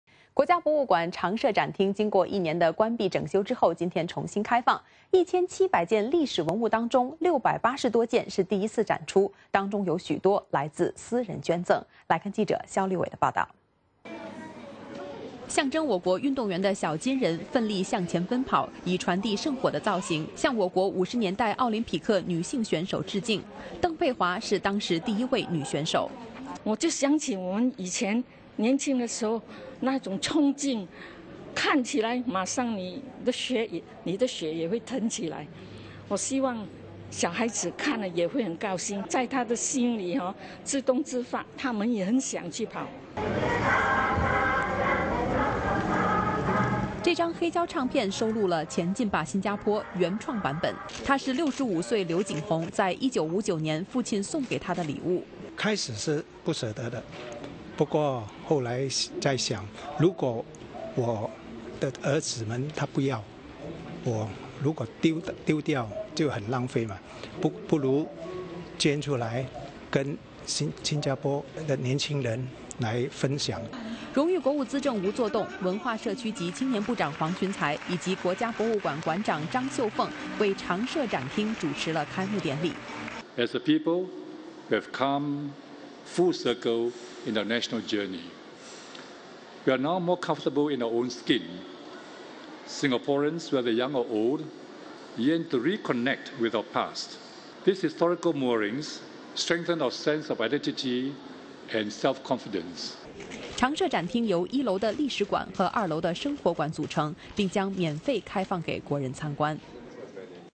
国家博物馆常设展厅 今天重新开放
国家博物馆常设展厅经过一年关闭整修后，今天重新开放。